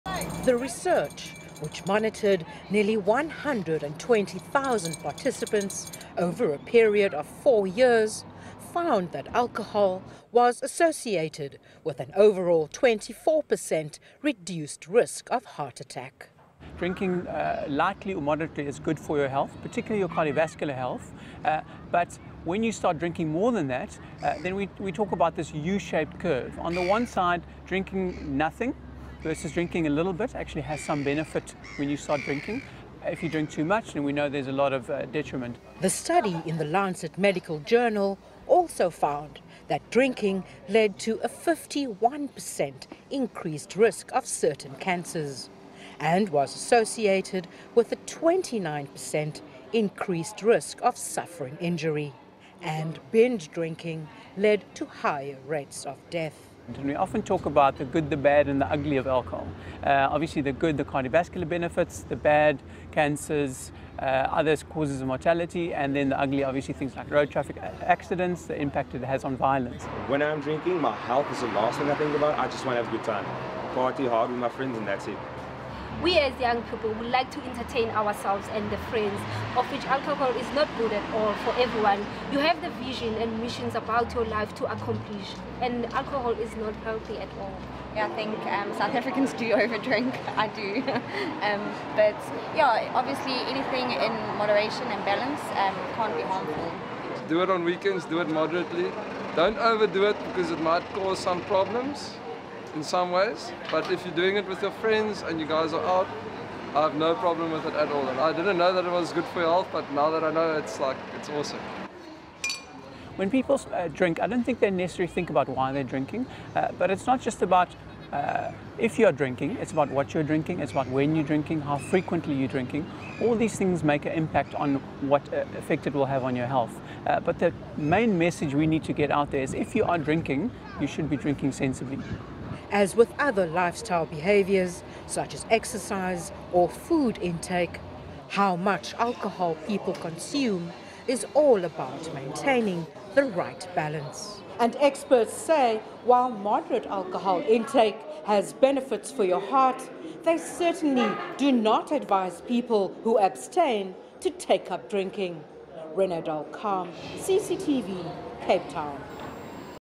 Alcohol consumption lowers risk of heart attacks
A recent study published in the Lancet Medical Journal suggests that alcohol consumption offers a certain level of protection against heart attack. Alcohol however also increases the risk of getting cancer according to the report. The study was carried out in 12 countries across the globe among them, South Africa and Zimbabwe